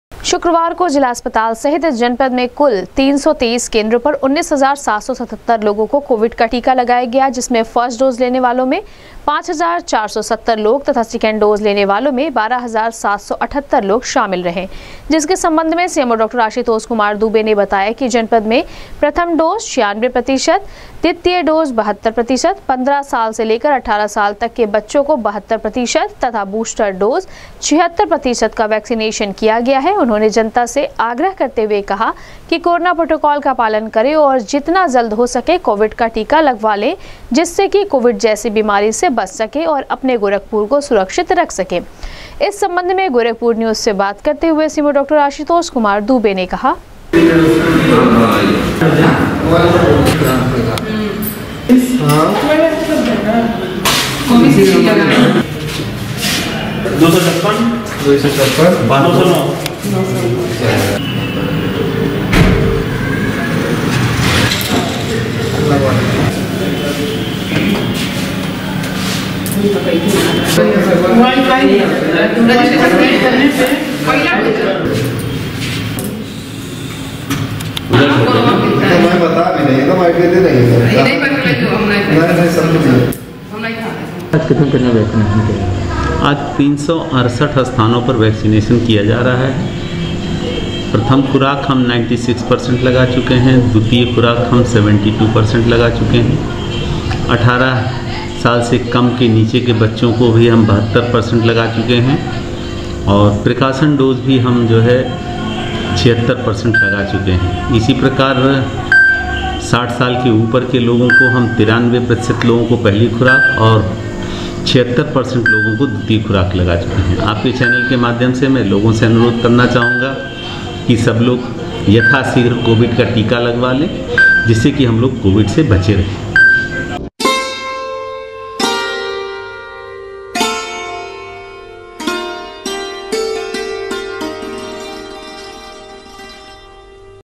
शुक्रवार को जिला अस्पताल सहित जनपद में कुल 323 केंद्रों पर 19,777 लोगो को किया गया कोविड का टीकाकरण
शुक्रवार को जिला अस्पताल सहित जनपद में कुल 323 केंद्रों पर 19,777 लोगो को किया गया कोविड का टीकाकरण , फस्र्ट डोज लेने वालों में 5,470 लोग तथा सेकेंड डोज लेने वालों में 12,778 लोग रहे शामिल,सीएमओ ने कहा, लगवा लें कोविडरोधी टीका, करें कोविड प्रोटोकॉल का पालन,लगाए मास्क, करें सोशल डिस्टेंसिंग का पालन